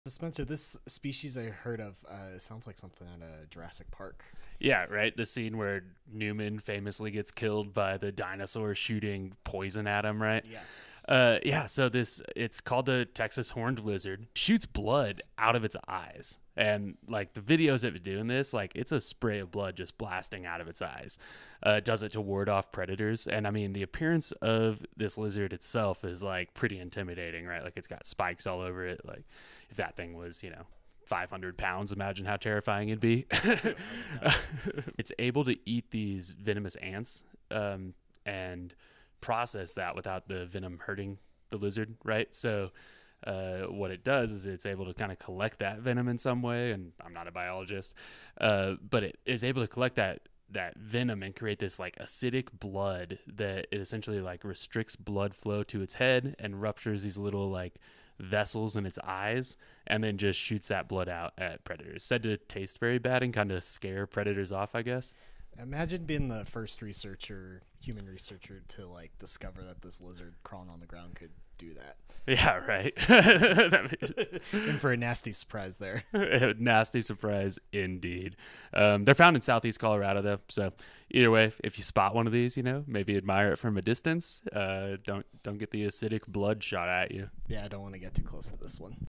Colorado Species Shoots Acidic Blood Out of Its Eyes
🦎 This species found in Colorado can shoot acidic blood out of its eyes!